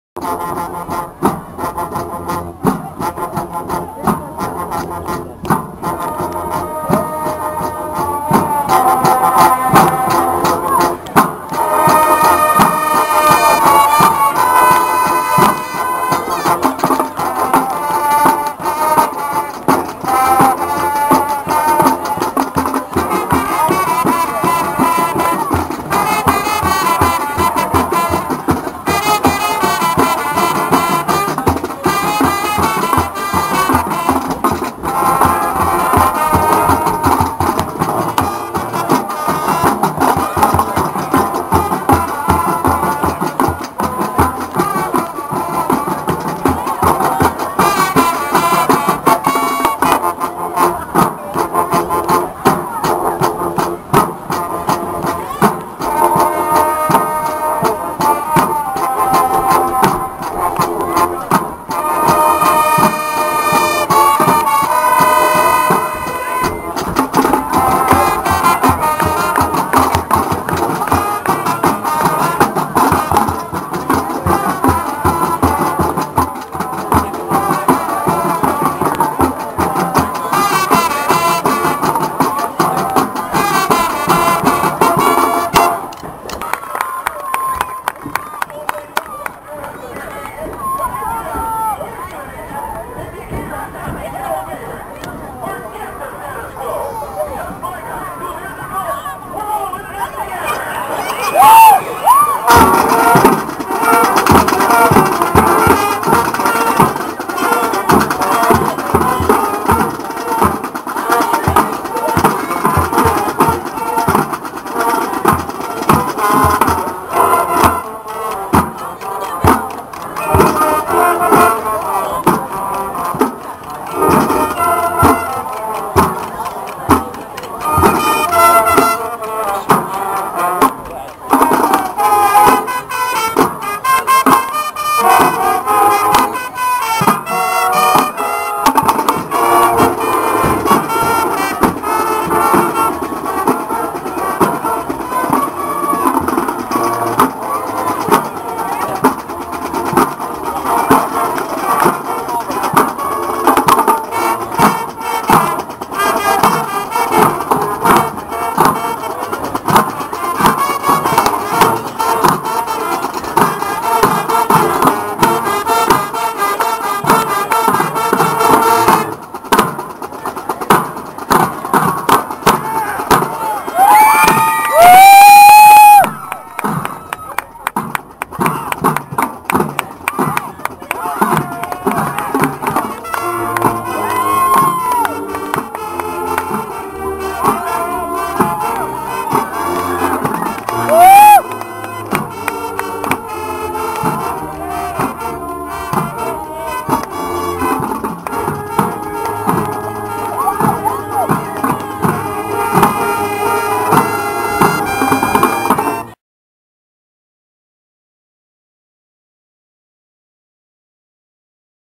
Band Show Premereing on Youtube in October!
St. Vincent St. Mary's 4th Annual Irish Band Show
Premieres Next Month on RittmanIndianBand Youtube Fan Site!
Also Premiereing Next Month Wayne County Fair - Band day N Bandarama at Wooster Triway Stadium...
Orrville Band Show: October 3rd | Our Band Show is: October 24 th..